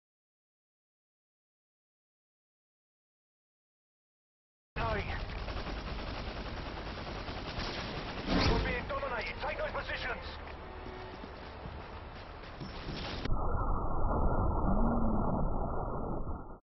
Grenade Fail
He goes for the triple with his AA-12 and Fails REALLY badly